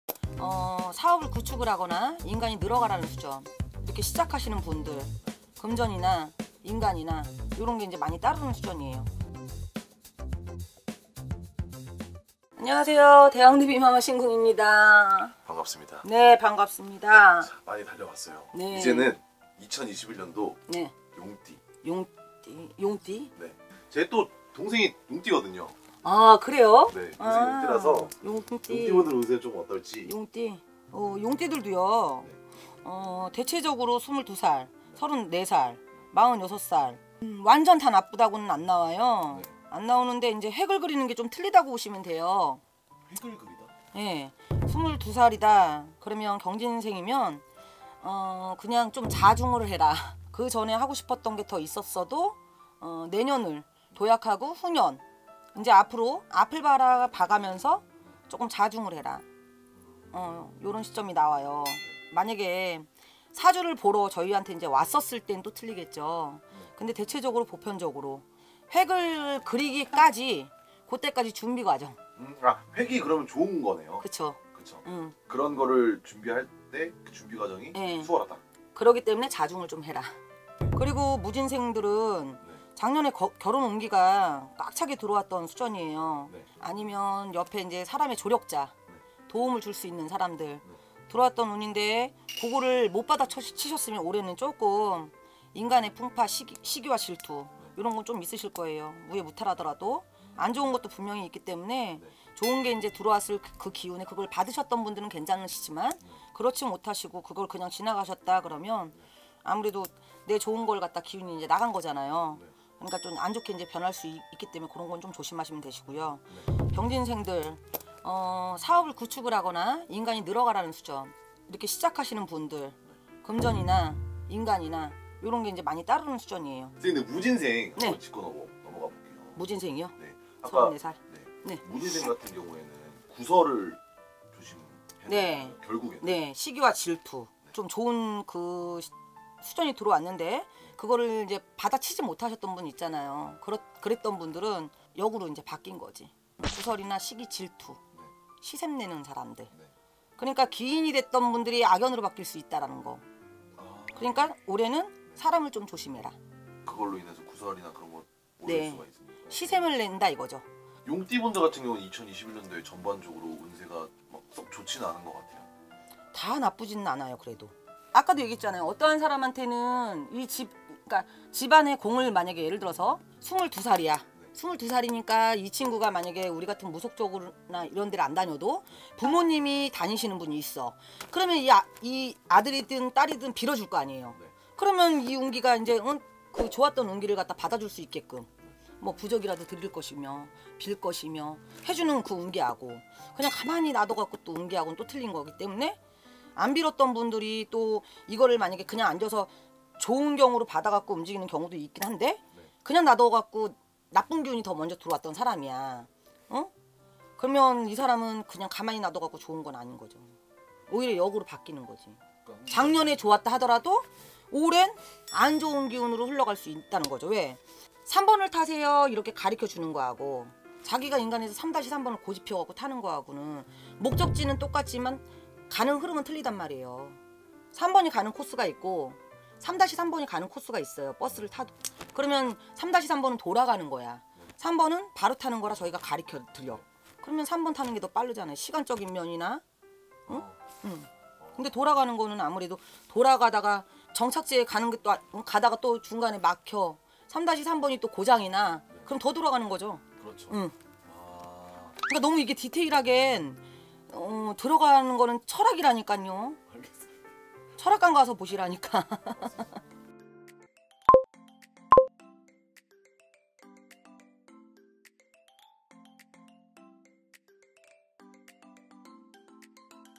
[2021년 운세] 용띠 신년운세 '금전운이 따라오는 용띠' (22세,34세,46세,58세) [경기광주점집 대비마마]
자막

★대표블로그★
-올마이티TV